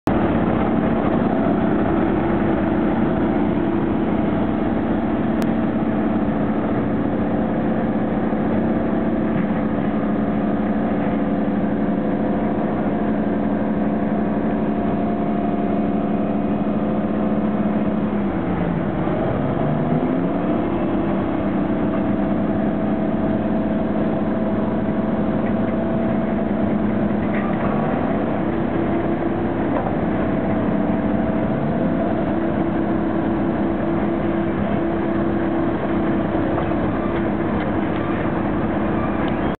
2010 HCEA Convention
Earl Fitzgerald feeds the shovels with his 1964 Euclid TC-12 dozer at the HCEA convention in Richmond, RI Sept 10-12